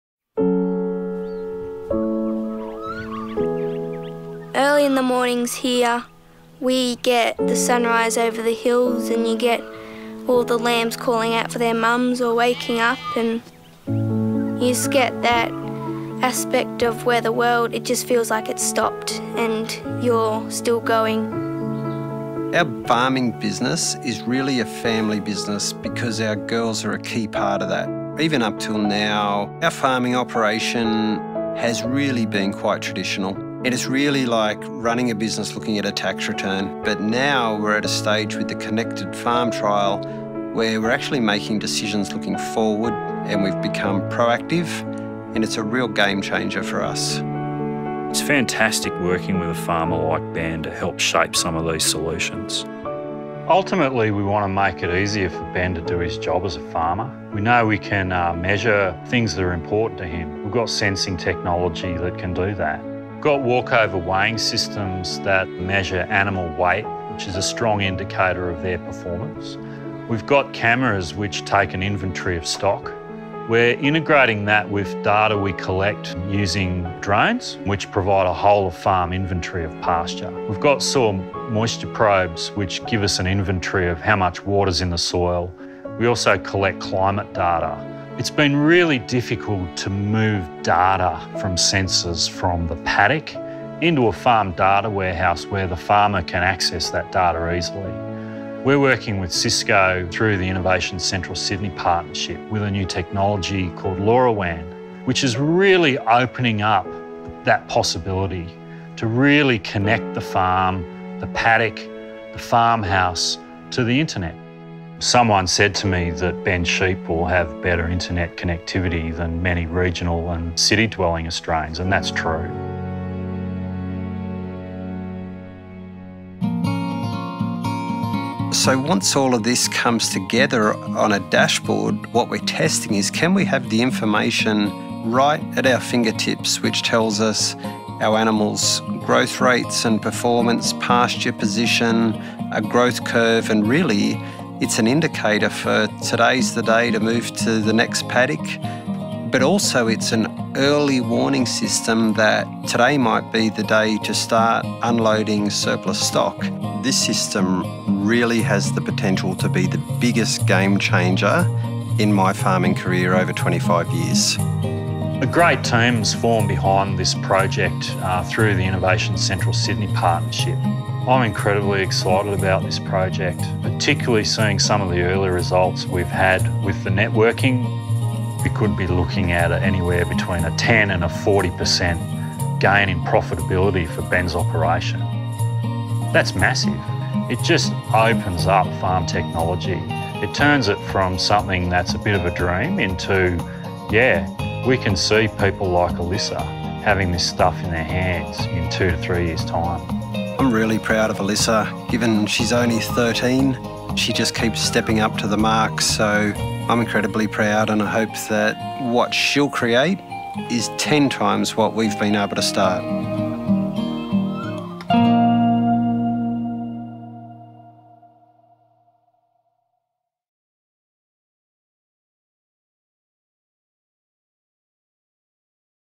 Big data creates savings for Molong farmer
Innovation Central Sydney Video about the Farm Decision Technologies project. The project is a partnership between the NSW Department of Primary Industry (DPI)  and Bralca Pty Ltd and is  supported by Innovation Central.  Innovation Central is a connected tech laboratory provided by CISCO, NSW DPI, CSIRO, NSWFarmers and the University of NSW.  Its aim is to accelerate adoption of digital technology in agribusiness.